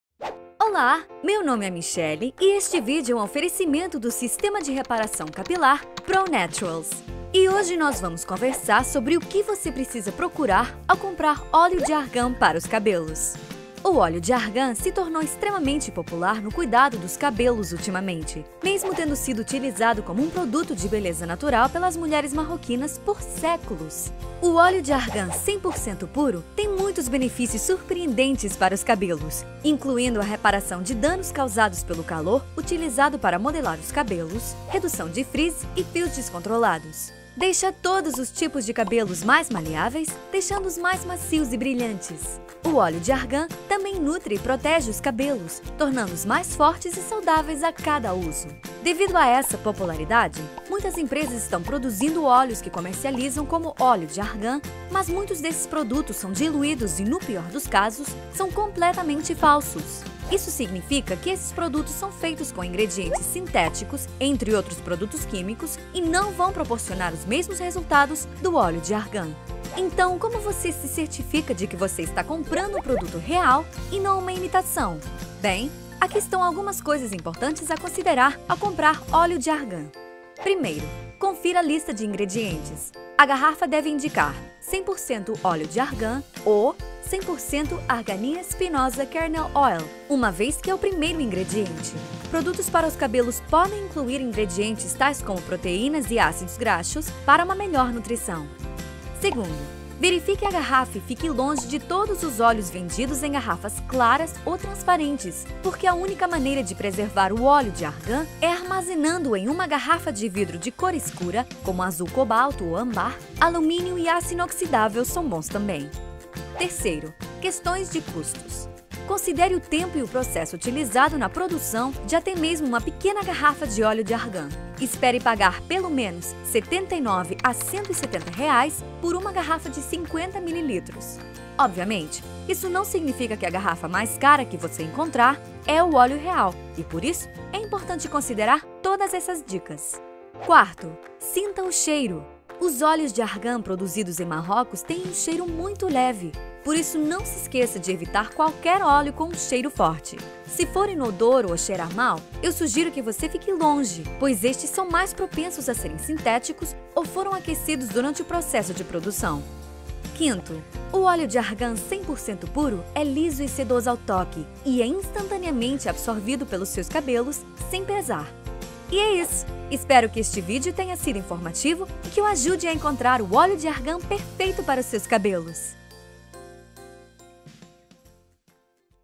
AVISO Oleo de Argan: 5 Aspectos a Considerar Antes de Comprar um Frasco
O óleo de argania tem muitos benefícios maravilhosos para o seu cabelo, porém com sua extrema popularidade, algumas empresas estão se aproveitando e produzindo um produto de qualidade inferior, adicionando substâncias para dar volume ou diluindo o óleo, reduzindo assim os verdadeiros benefícios do óleo de argania para o cabelo.

Neste pequeno vídeo, explicamos o que você precisa procurar e o que você deve evitar ao comprar o óleo de argania marroquino, para que você possa ter certeza de que está recebendo o melhor e não desperdiçando seu dinheiro em produtos falsificados ou inferiores.

O óleo de argania pode mudar de forma positiva e radical a aparência ou o toque de seu cabelo, e é benéfico para todos os tipos e texturas de cabelo. Vamos garantir que você obtém o que merece de seu óleo de argania, assista a este vídeo e fique informado!